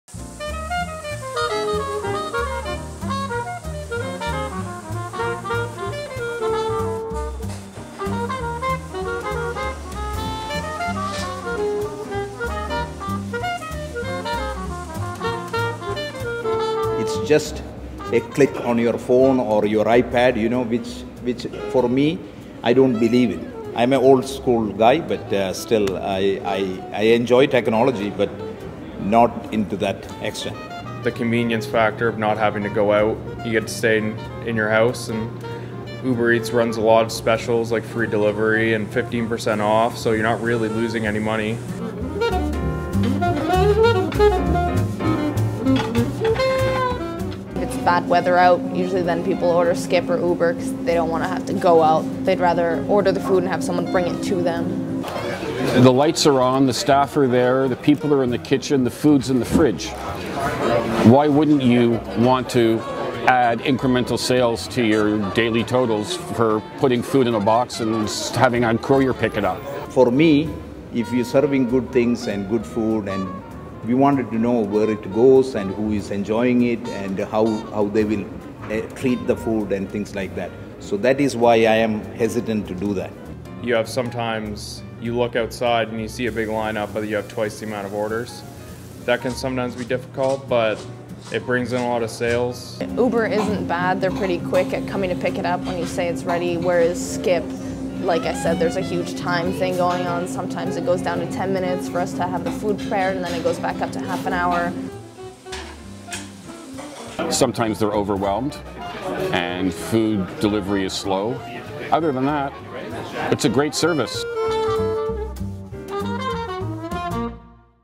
What restaurateurs says about food delivery
We asked four local restaurateurs how they felt about food delivery services like Uber Eats and Skip the Dishes.